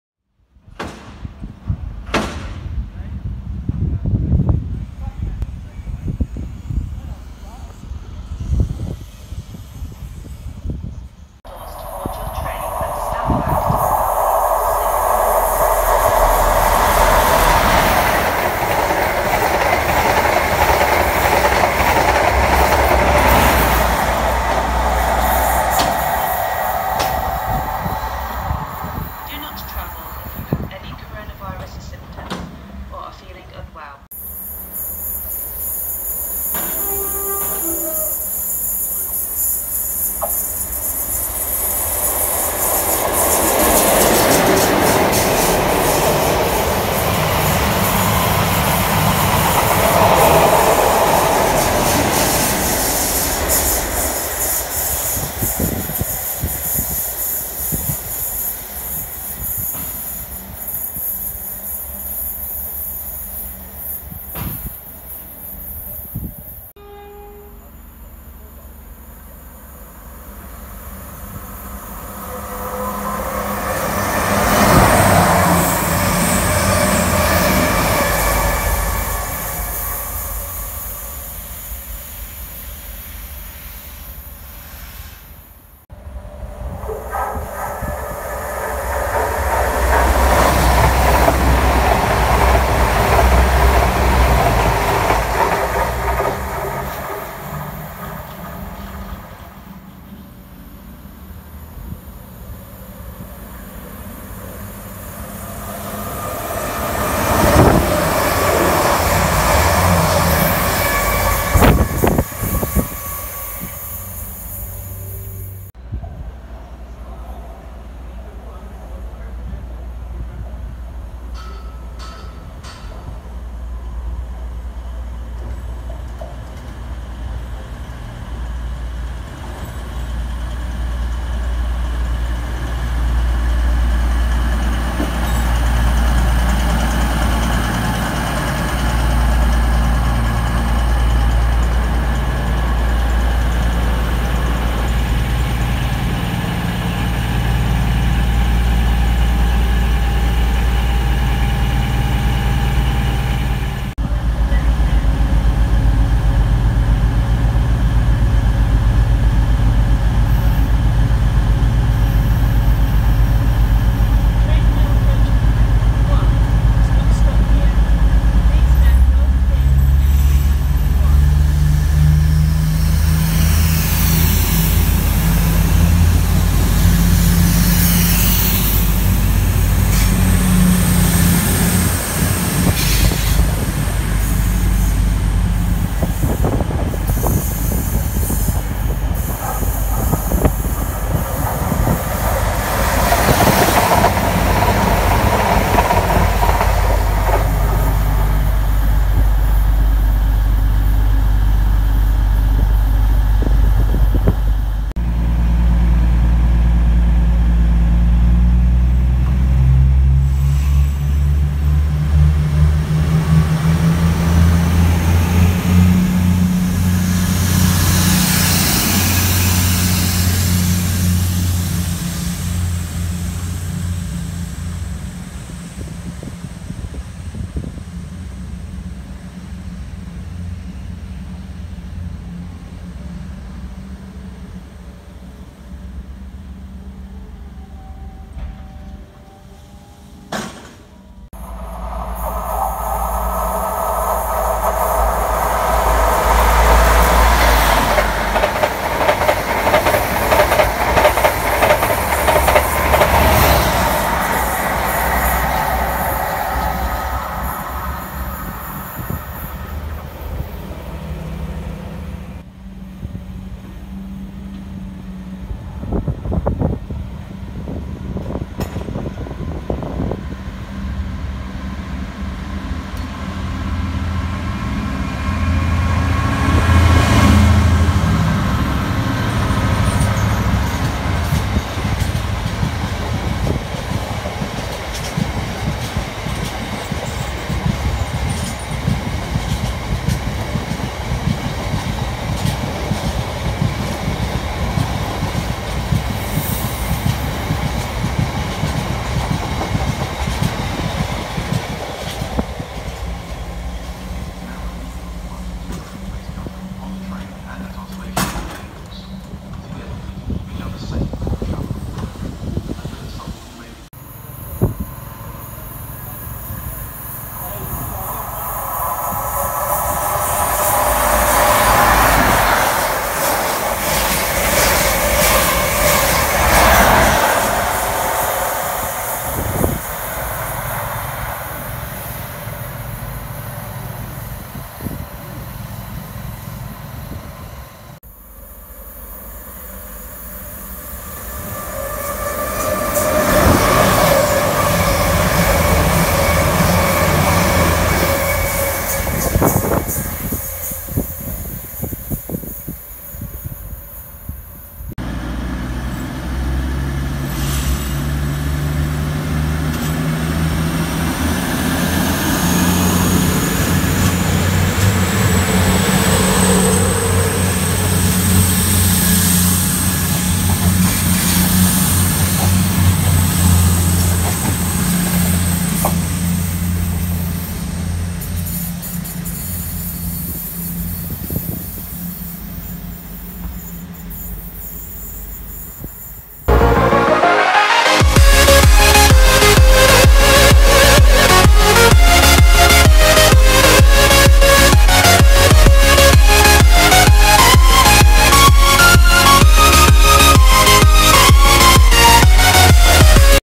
Trains at Wellingborough STN (18/08/20)
Wellingborough railway station is located the market town of Wellingborough in Northamptonshire, England. It lies on the Midland Main Line, it is 65 miles (104 km) from London St. Pancras. East Midlands Trains (EMT) operate the station and run most of its services using Meridian trains.

The station is the closest to the towns and large villages of Higham Ferrers, Irchester, Irthlingborough, Rushden and Wollaston although there is no direct link to any of the destinations from the station apart from Irthlingborough.

With Northamptonshire's history in the shoe industry, the film Kinky Boots filmed in the county and featured Wellingborough station but branded it as Northampton. In late 2009 Wellingborough was made a Penalty fare station by East Midlands Trains, which means valid ticket or Permit to travel must be shown when requested.
Name: Wellingborough
CRS Code: WEL
Location Code: 194000
Info Board Name: WELLINGBOROUGH
Address: Wellingborough station, Midland Road, Wellingborough, Northamptonshire
Postcode: NN8 1NA
Operator: East Midlands Railway
Opened: 1857
Number of platforms: 3
DfT Category: C2 (Large trunk station)
Network Rail Route: London North East and East Midlands
Strategic Route Code: I.02
Strategic Route Line: Bedford - Corby / Nottingham
Information gathered from:
Realtime Trains -